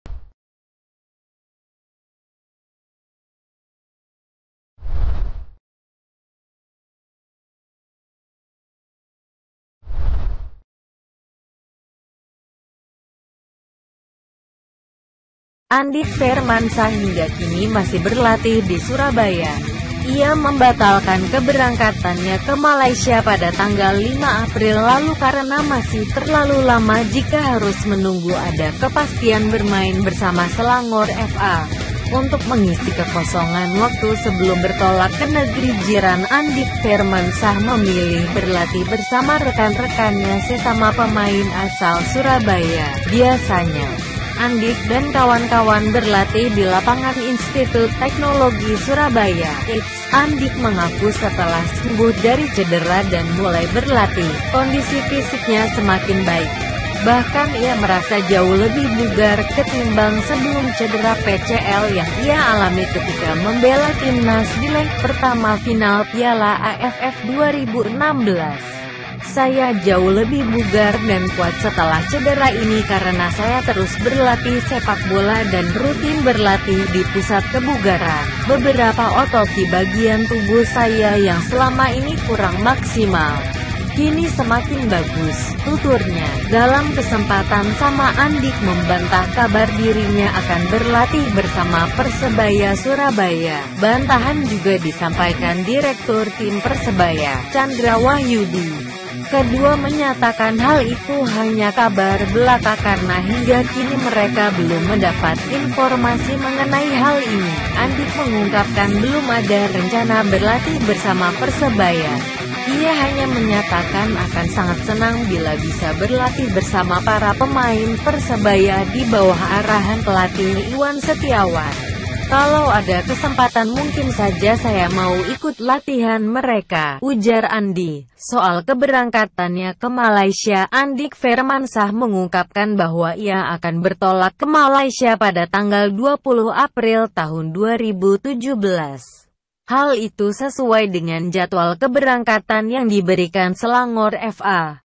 Begini Pengungkapan Andik Vermansah Soal Ikuti Latihan Bersama Persebaya
Andik Vermansah hingga kini masih berlatih di Surabaya. Ia membatalkan keberangkatannya ke Malaysia pada 5 April lalu karena masih terlalu lama jika harus menunggu ada kepastian bermain bersama Selangor FA.
Untuk mengisi kekosongan waktu sebelum bertolak ke Negeri Jiran, Andik Vermansah memilih berlatih bersama rekan-rekannya sesama pemain asal Surabaya. Biasanya, Andik dan kawan-kawan berlatih di Lapangan Institut Teknologi Surabaya (ITS).
Andik mengaku setelah sembuh dari cedera dan mulai berlatih, kondisi fisiknya semakin baik. Bahkan ia merasa jauh lebih bugar ketimbang sebelum cedera PCL yang ia alami ketika membela Timnas di leg pertama final Piala AFF 2016.